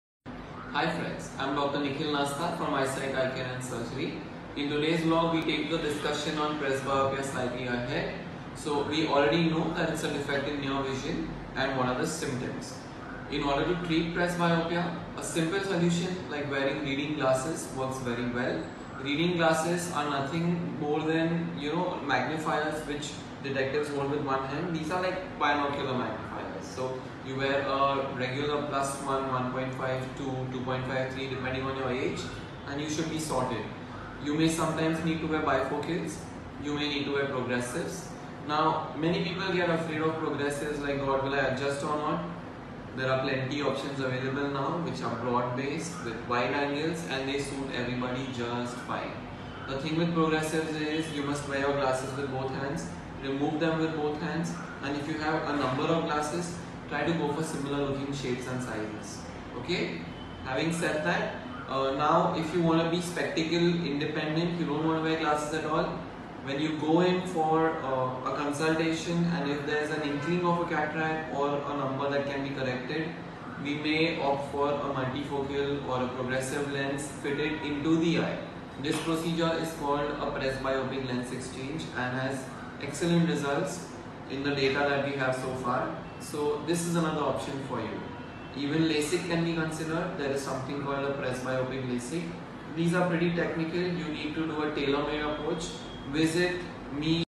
Presbyopia Signs and Symptoms
How to treat presbyopia ( Farsightedness ) ?
Watch the third video of Dr. Nikhil Nasta on presbyopia, its Signs, and Symptoms.